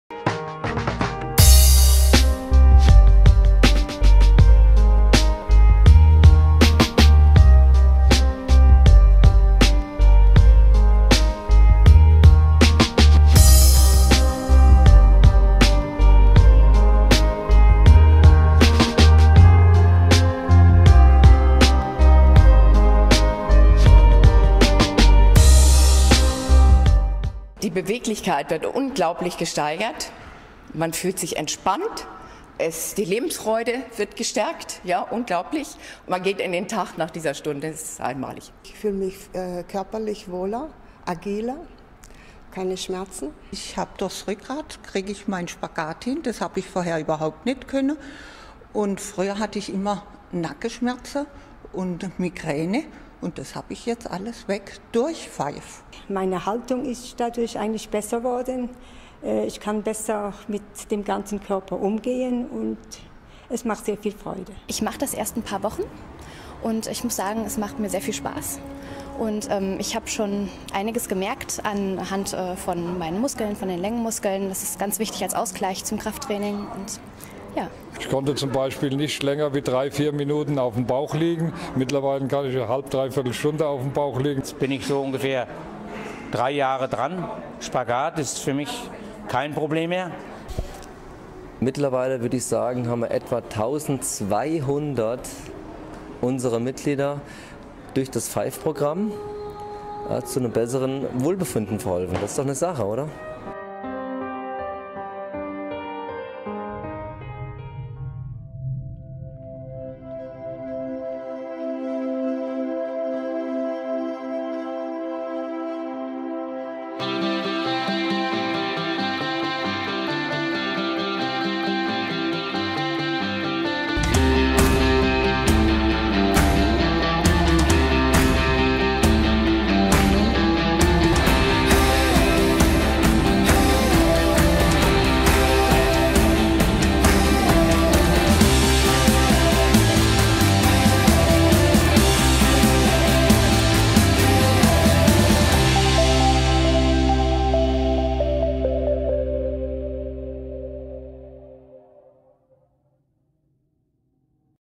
five® - Das Rücken- & Gelenkkonzept im POM Weimar
Das  five-Konzept ist aus der Überlegung heraus entstanden, dass fünf Grundübungen notwendig sind, um den ganzen Körper voll funktionsfähig, geschmeidig und beweglich zu machen und zu halten.
five® wirkt als Gegenpol zum Arbeitsalltag und arbeitet deshalb schwerpunktmäßig mit Rückwärtsbewegungen. Zehn speziell dafür entwickelte Geräte ermöglichen ein geführtes und kontrolliertes Training auch in Eigenregie. Entsprechende Kurse ermöglichen Anfängern einen schnellen und fehlerfreien Einstieg.

Das  five-Kursprogramm ist ein Präventionskurs nach §20 SGB V. Das heißt, dass Deine Krankenkasse mindestens 75% der Kursgebühren übernimmt. Natürlich helfen wir Dir bei den Formalitäten. Ganz Kurs & schmerzlos.